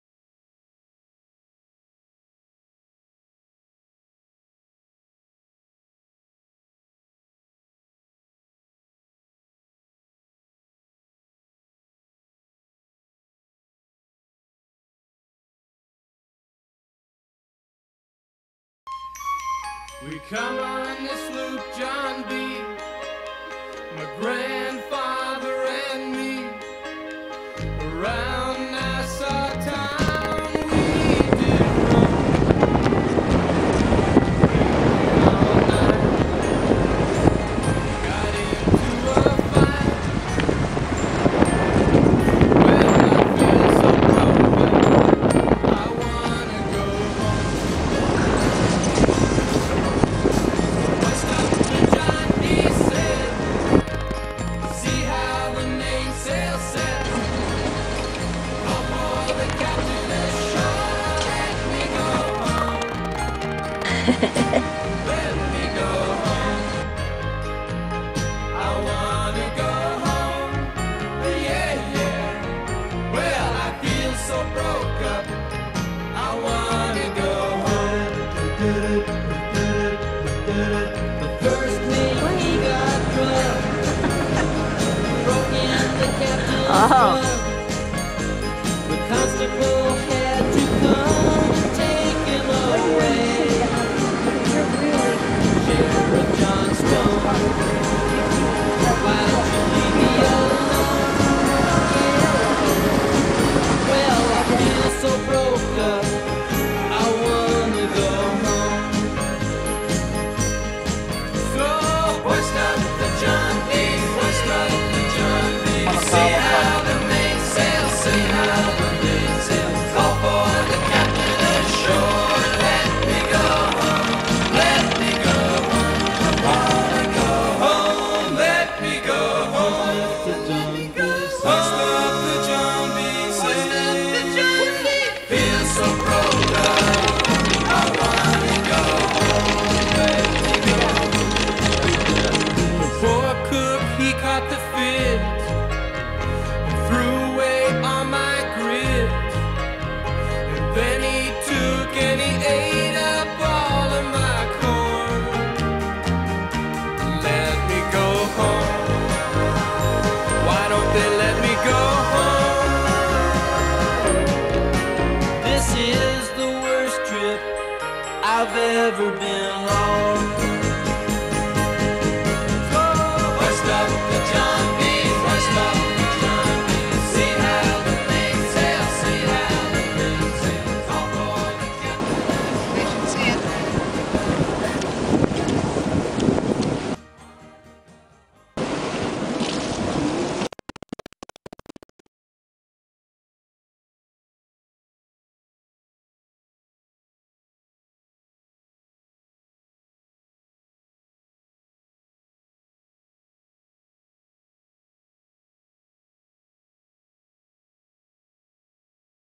The Stowaways
A short film about Barbara and Marilynn, two stowaways aboard the yacht RobinLeigh.